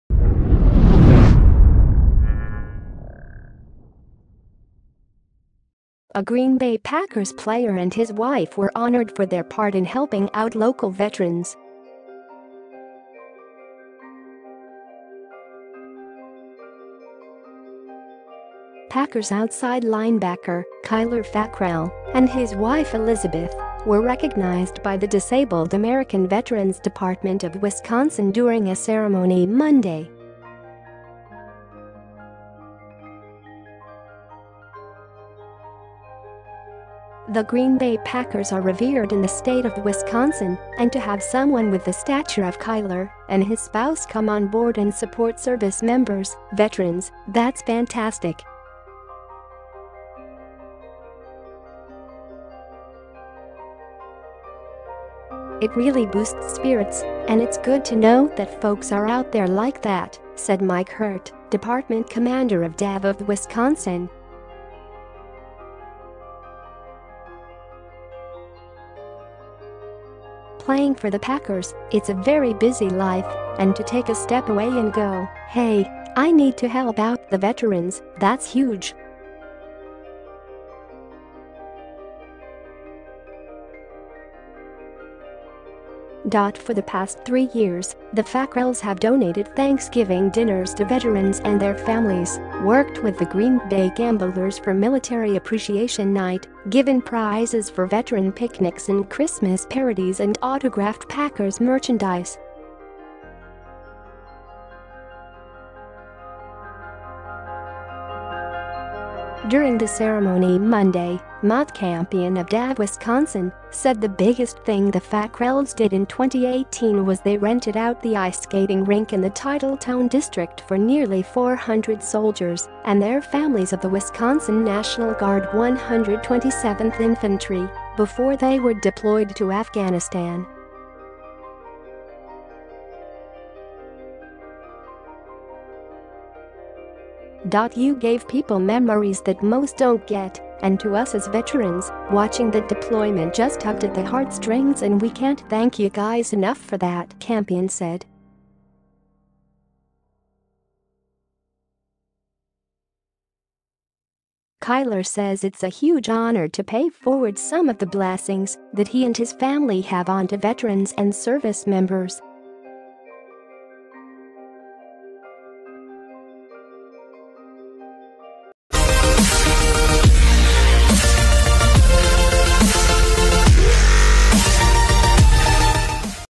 Packers' Fackrell honored for work with veterans
A Green Bay Packers player and his wife were honored for their part in helping out local veterans.

Packers outside linebacker, Kyler Fackrell, and his wife Elizabeth, were recognized by the Disabled American Veterans Department of Wisconsin during a ceremony Monday.

“The Green Bay Packers are revered in the state of Wisconsin and to have someone with the stature of Kyler and his spouse come on board and support service members, veterans, that’s fantastic. It really boosts spirits and it’s good to know that folks are out there like that," said Mike Hert, department commander of DAV of Wisconsin. "Playing for the Packers, it’s a very busy life and to take a step away and go, hey, I need to help out the veterans, that’s huge.”

For the past three years, the Fackrell's have donated Thanksgiving dinners to veterans and their families, worked with the Green Bay Gamblers for Military Appreciation Night, given prizes for veteran picnics and Christmas parities and autographed Packers merchandise.
During the ceremony Monday, Matt Kampien of DAV Wisconsin, said the biggest thing the Fackrell's did in 2018 was they rented out the ice skating rink in the Titletown District for nearly 400 soldiers and their families of the Wisconsin National Guard 127th Infantry before they were deployed to Afghanistan.

“You gave people memories that most don’t get and to us as veterans, watching that deployment just tugged at the heartstrings and we can’t thank you guys enough for that," Kampien said.

Kyler says it's a huge honor to pay forward some of the blessings that he and his family have onto veterans and service members.